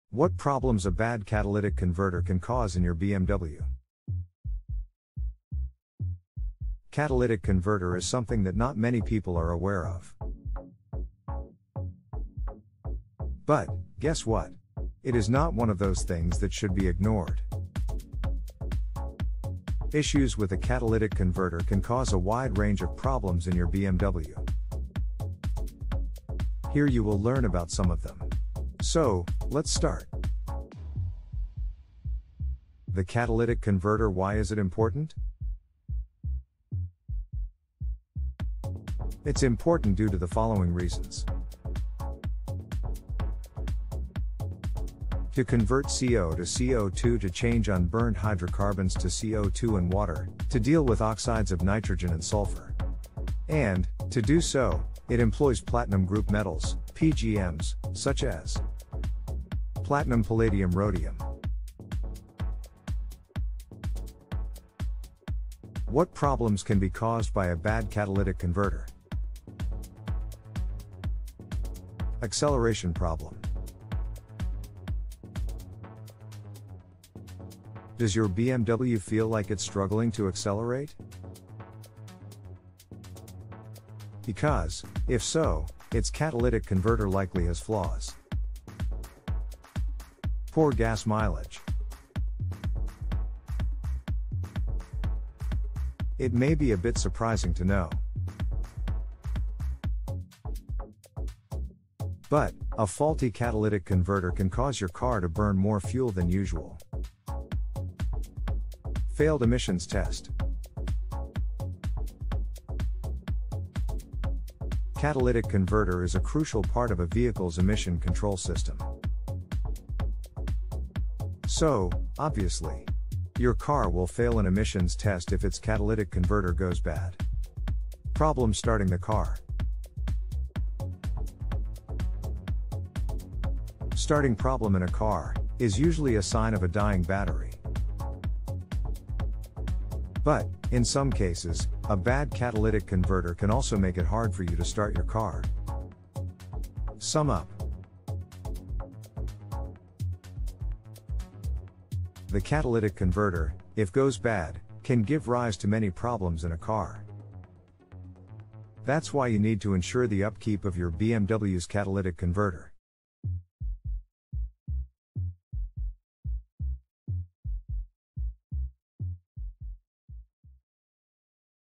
What Problems A Bad Catalytic Converter Can Cause In Your BMW
Issues with the catalytic converter can result in all sorts of drivability problems- including acceleration issues. Therefore, if you suspect that there is something off with your BMW's catalytic converter, get it tested by a mechanic right away.